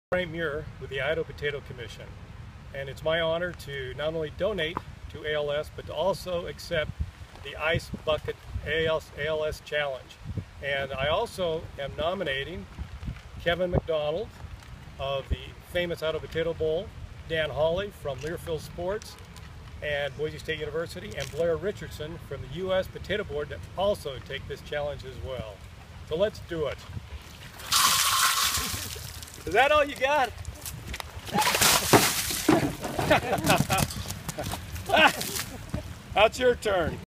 Frank Muir's ALS
Frank Muir, President/CEO of the Famous Idaho Potatoes accepted the ALS IceBucketChallenge and handled it like a champ!

Blair Richardson, Dan Holly and Kevin McDonald... It's your turn!!! You have officially been IceBucketChallenged!